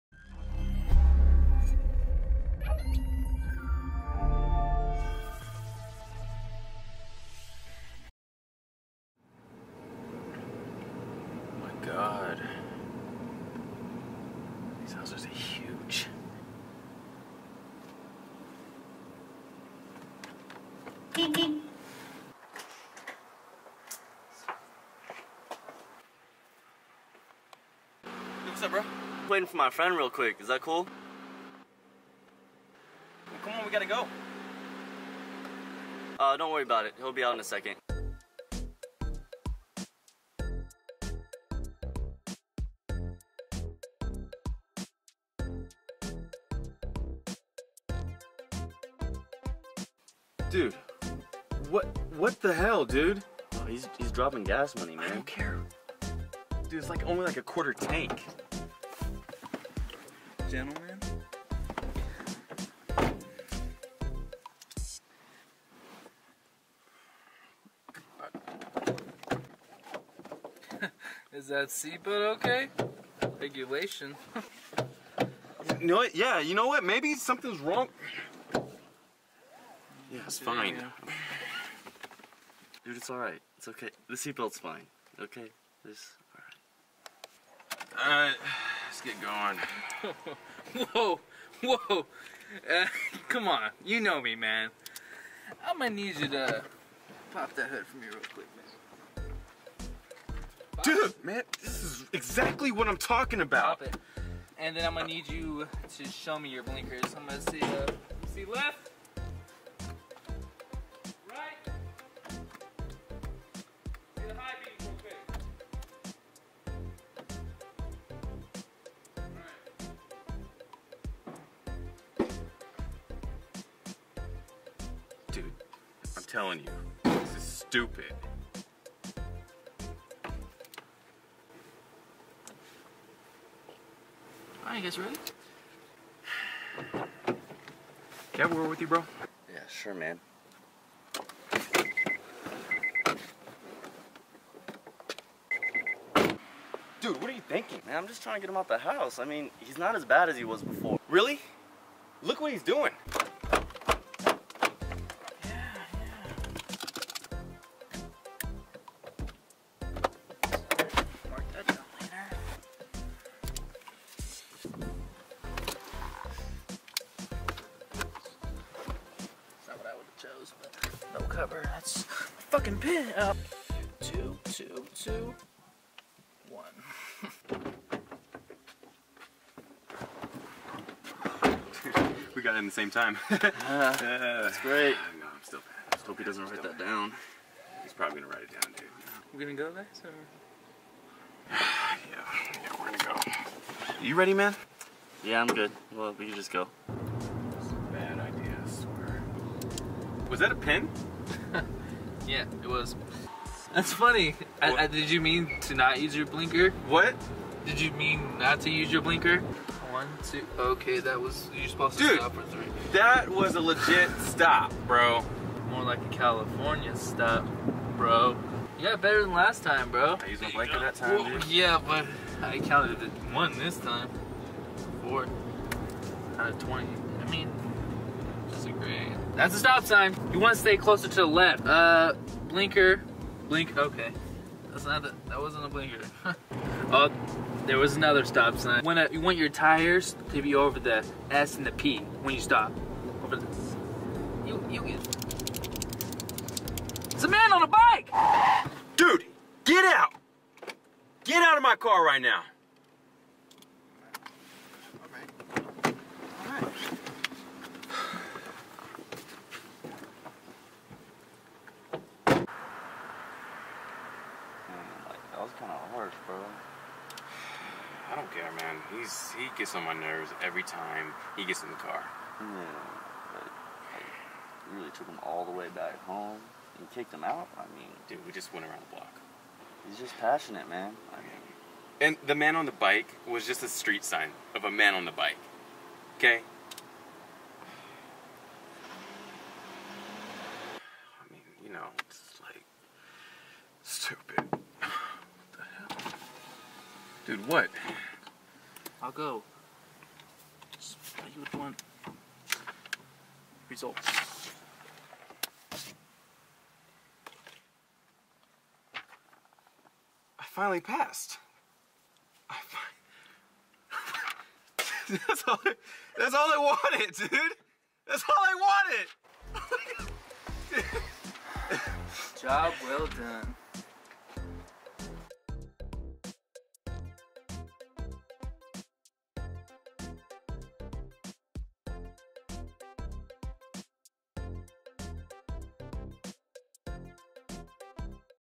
Backseat Driver |Skit
Willowbrook Films Presents: Backseat Driver

Two friends are in for a little more than they can handle when they invite their more auto-aware friend with them on a ride.

Staring:
Benjamin Ramirez
Derek Denten
Marjonni Brown